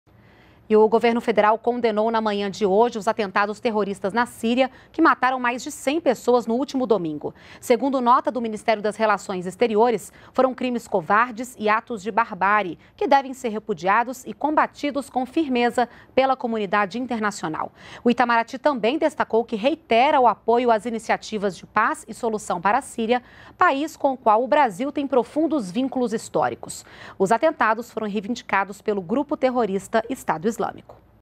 Em nota, governo brasileiro condena atentados terroristas na Síria
NBR NOTÍCIAS - 22.02.16: O governo federal condenou os atentados terroristas ocorridos na Síria no último domingo (21), que resultaram na morte de mais de 100 pessoas. Segundo nota do Ministério das Relações Exteriores, foram crimes covardes e atos de barbárie que devem ser repudiados e combatidos com firmeza pela comunidade internacional. O Itamaraty também destacou que reitera o apoio às iniciativas de paz e solução para a Síria, país com o qual o Brasil tem profundos vínculos históricos.